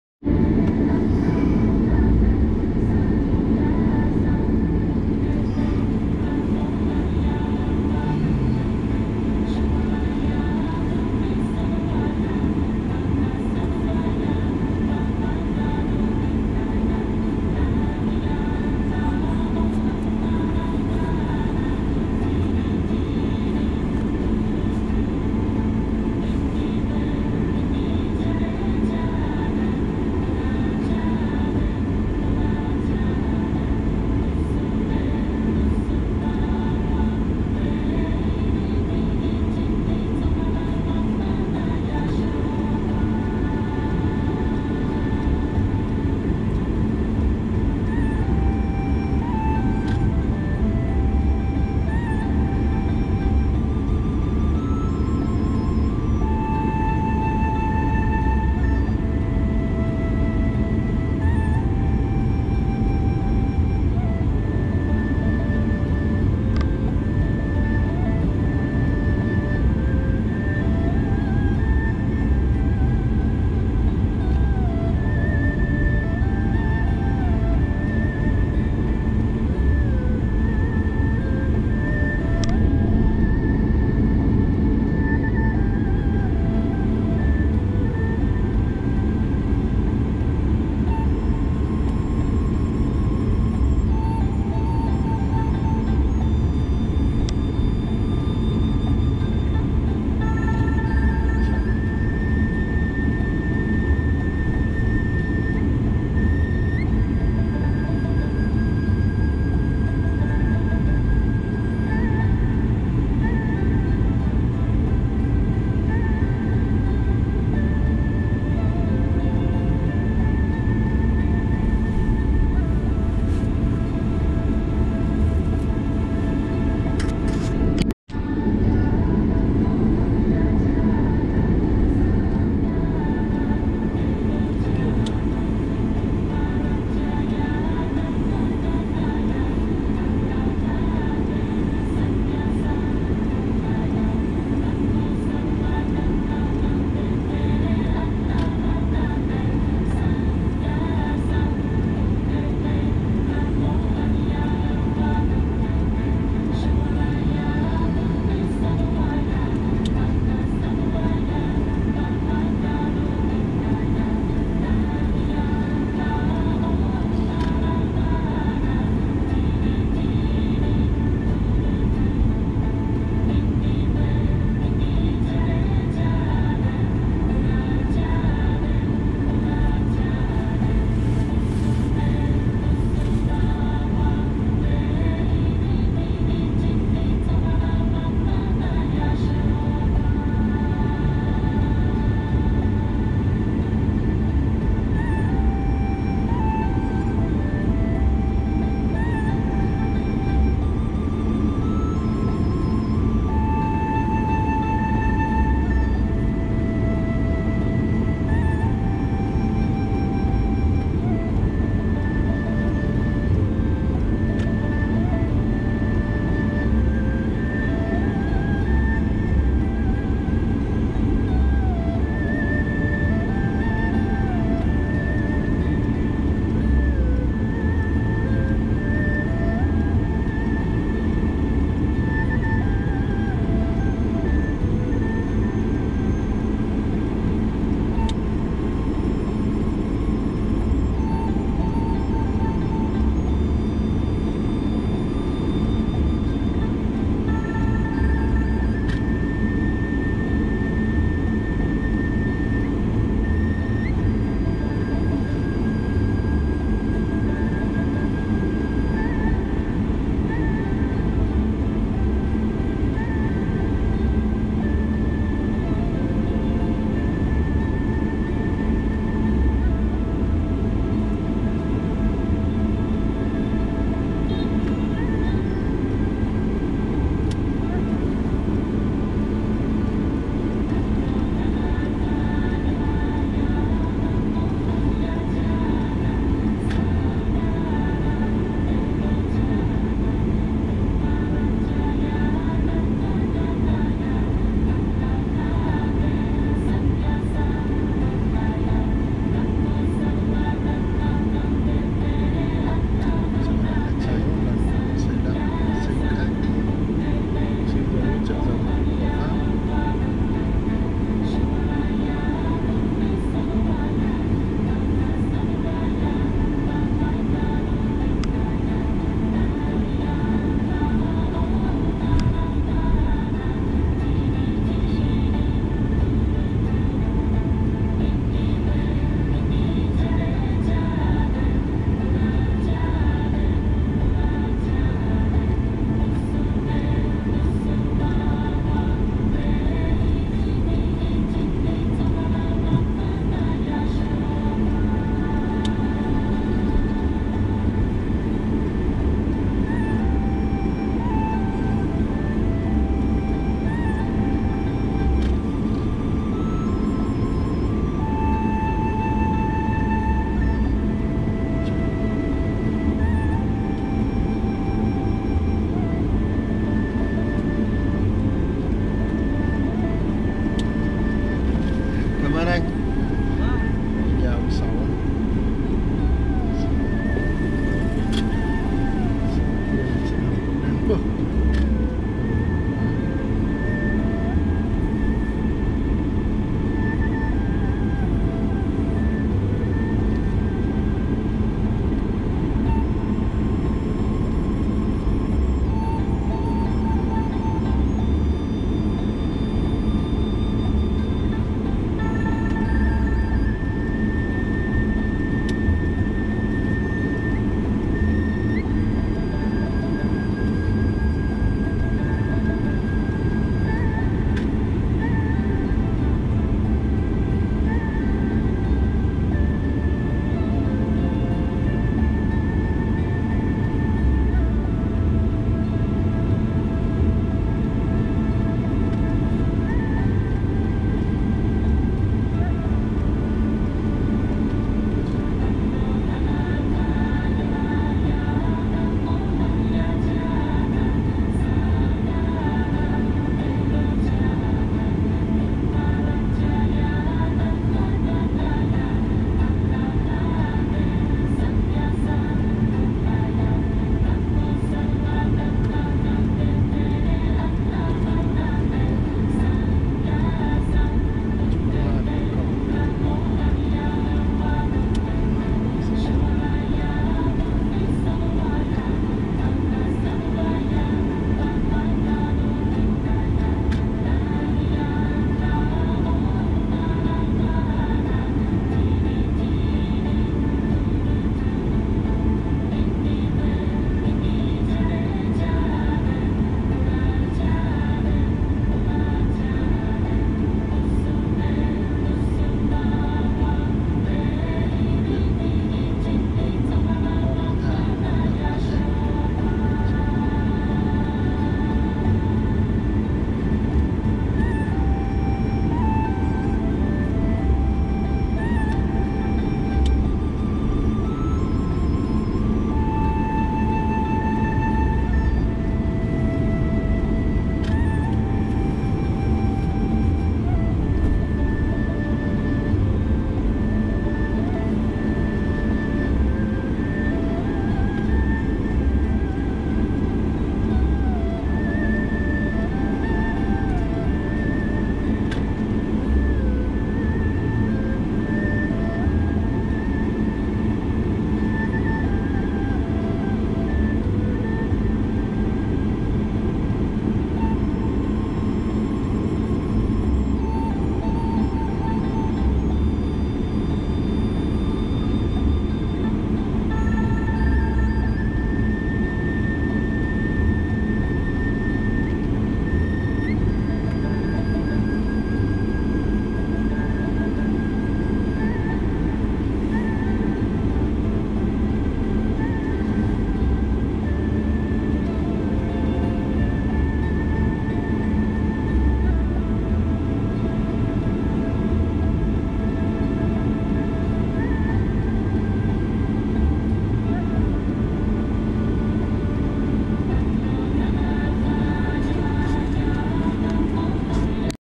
Nam Định ơi (050)
Đường Phố trong thành phố Nam Định, tháng 5 năm 2019

NXHYT/20192005/10